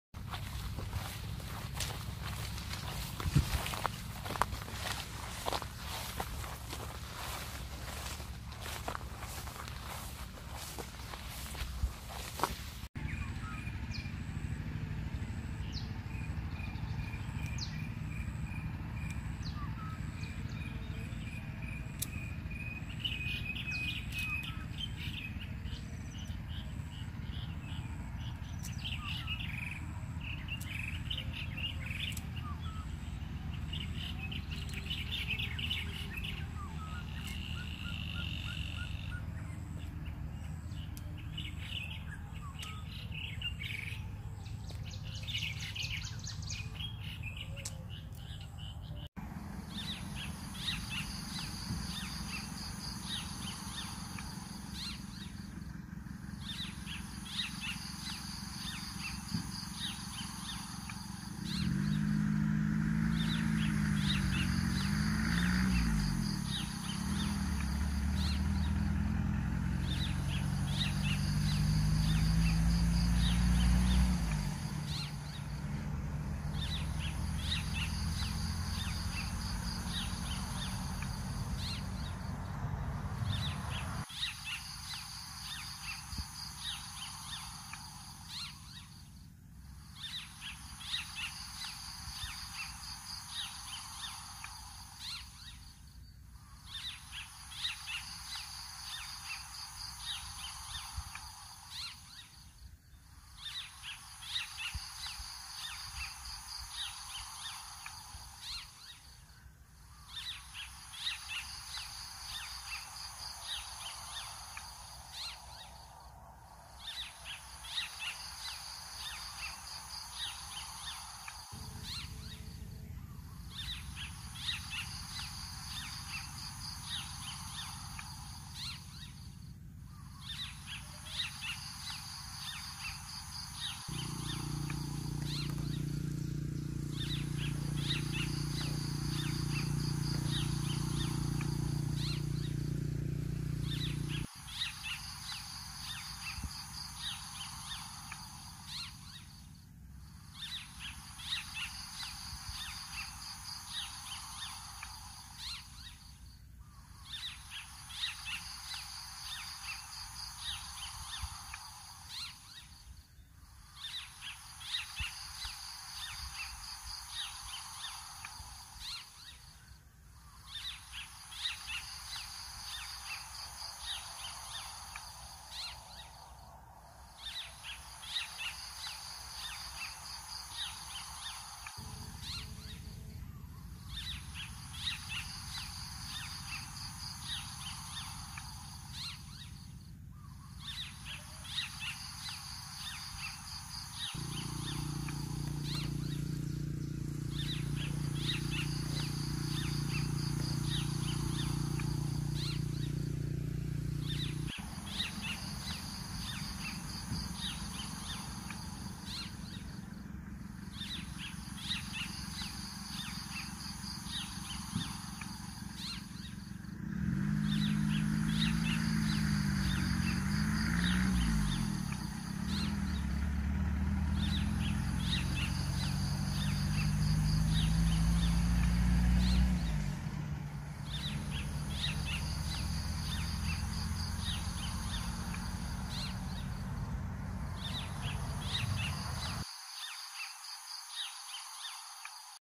The best bird song, Crested treeswift (Hemiprocne coronata) by bird guide
The crested treeswift is a kind of tree swift. It was for some time considered the same species as its eastern relative, the grey-rumped treeswift, but they do not interbreed where their ranges overlap. It is distinct in flight, with long, bowed wings and a deeply forked tail that usually appears narrow and pointed.